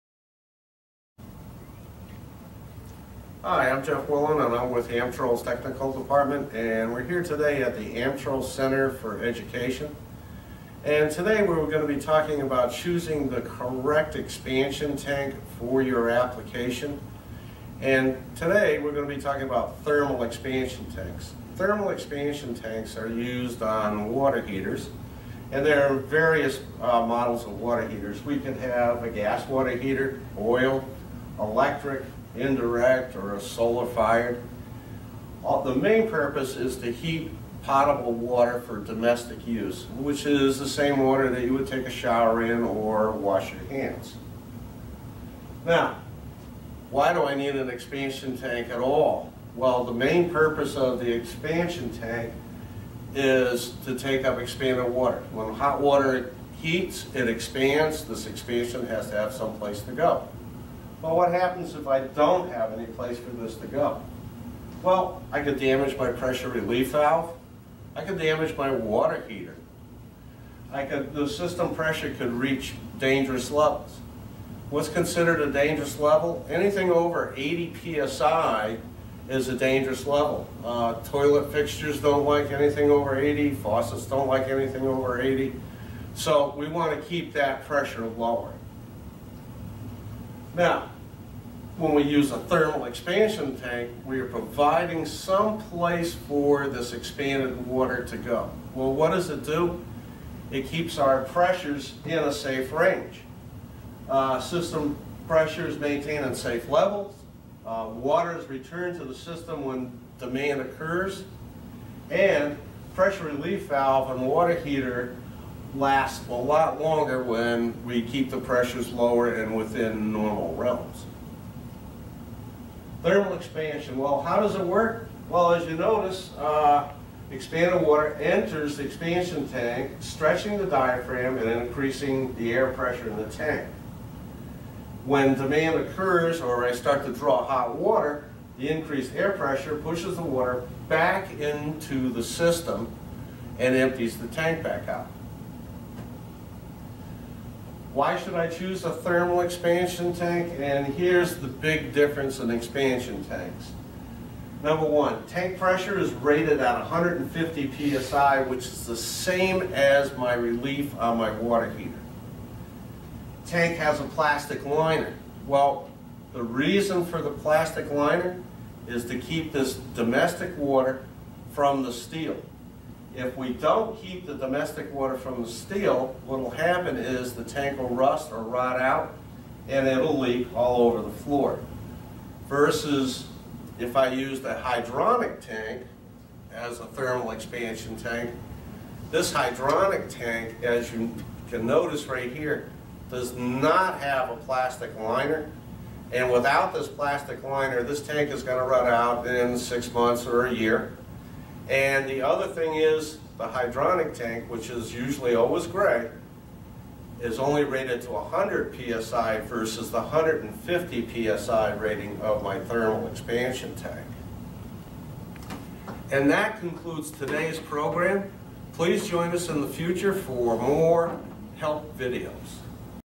Choosing The Right Expansion Tank For Your Application - Part 1
Jeff Wellen from Amtrol's Technical Department explains choosing the correct Thermal Expansion Tank in Part 1.